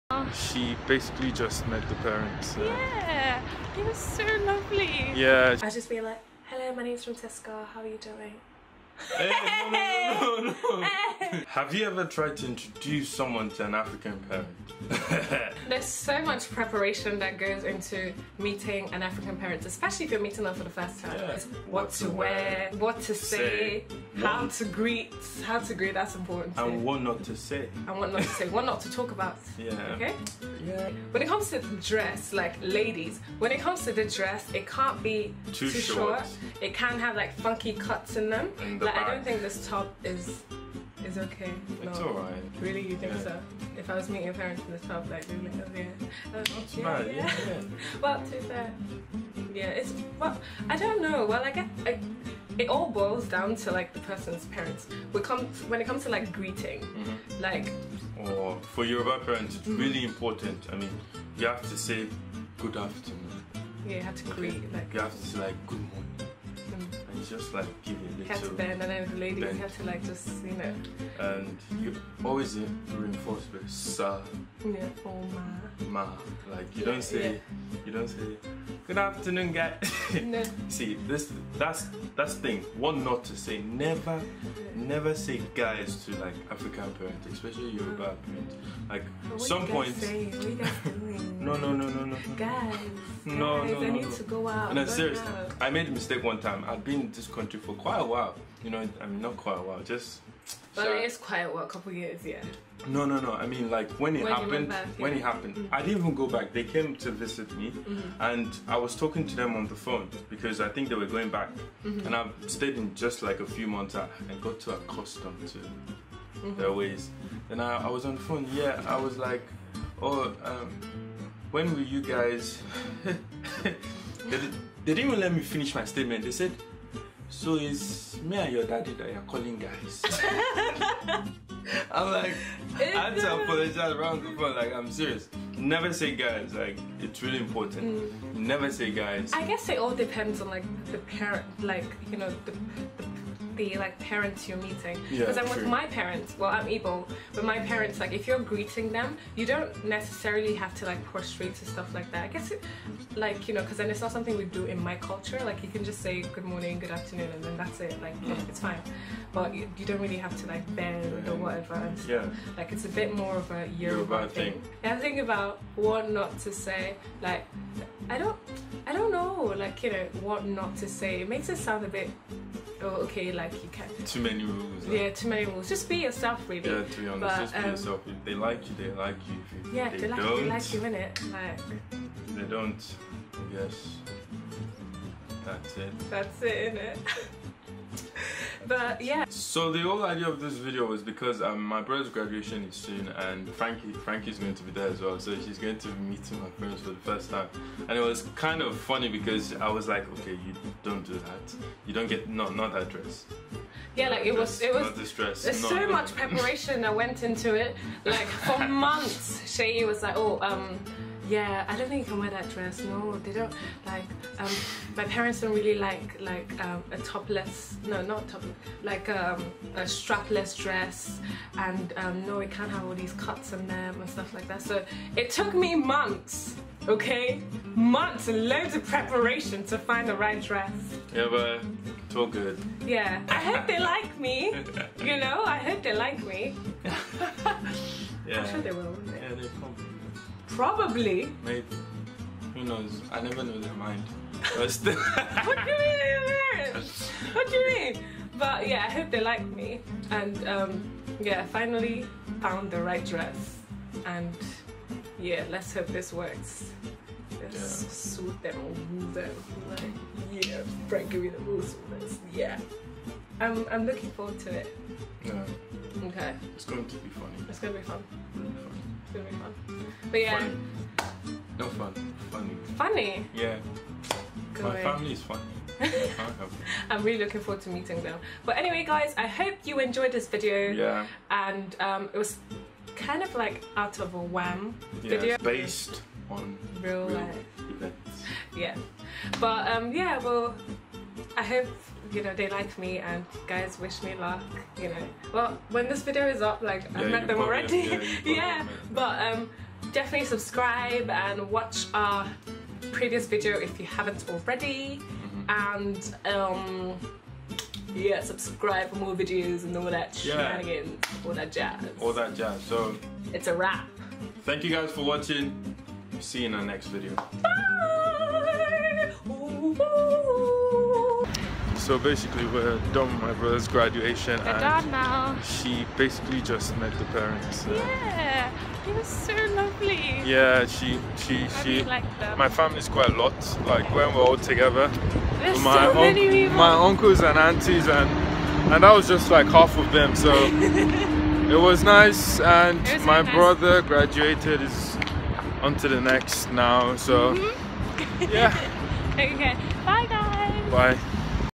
MEETING THE PARENTS FOR THE FIRST TIME | FRANKIE 'N' SEYI
Frankie meets Seyi's parents for the first time.Based on real life events. We hope you enjoy :)